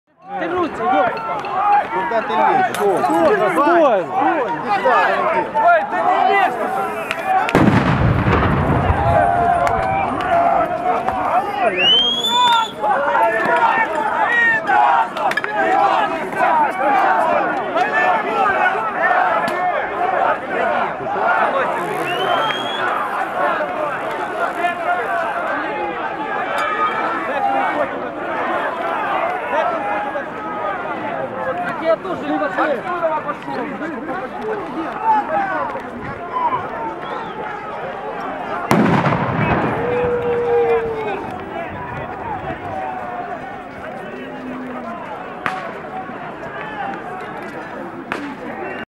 Mykolaiv Residents Destroy Russian Mercenaries' Camp In Ukraine
With credits to HukBecTu
The present material selected, adapted & published by EMPR #1 independent citizen media about Ukraine |